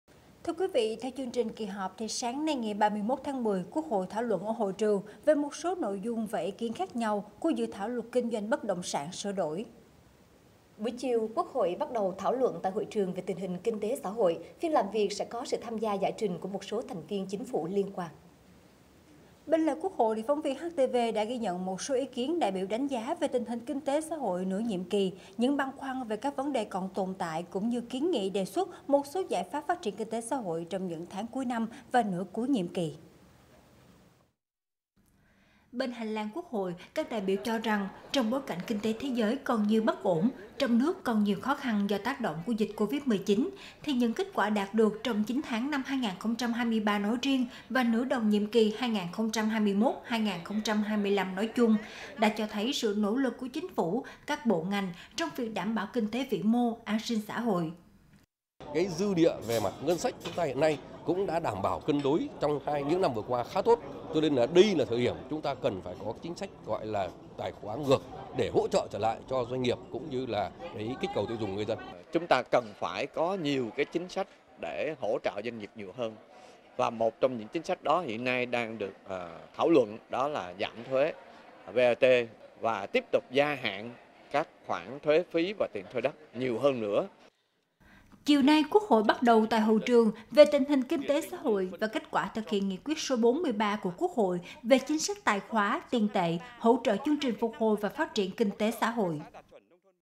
ĐẠI BIỂU QUAN TÂM NHIỀU VẤN ĐỀ KINH TẾ- XÃ HỘI
Bên lề quốc hội, phóng viên HTV đã ghi nhận một số ý kiến đại biểu đánh giá về tình hình kinh tế xã hội nửa nhiệm kỳ, những băn khoăn về các vấn đề tồn tại cũng như kiến nghị, đề xuất một số giải pháp phát triển kinh tế - xã hội trong những tháng cuối năm và nửa cuối nhiệm kỳ.